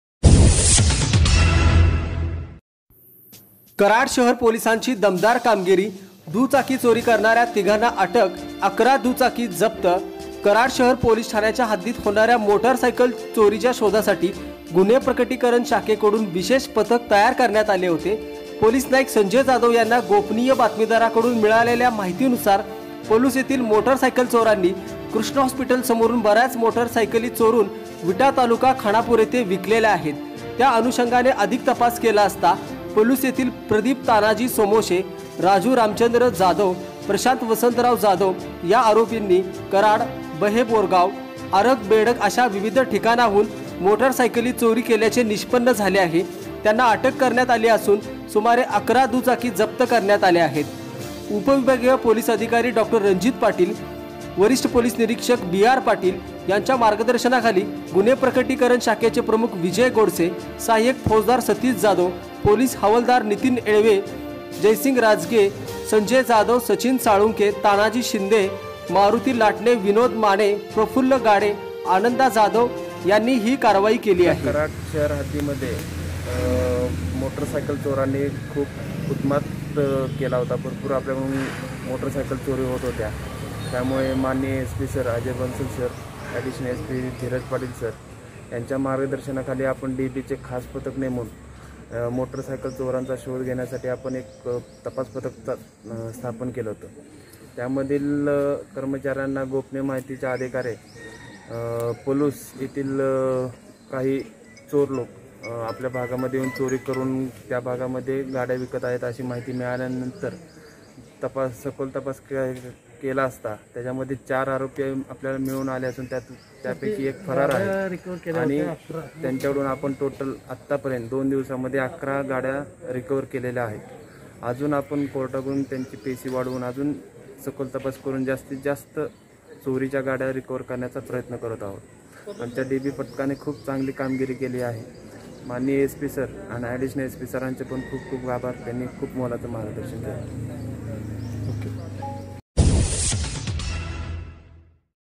दुचाकी चोरी करणाऱ्या तिघांना अटक, अकरा दुचाकी जप्त
कराड शहर पोलिसांची दमदार कामगिरी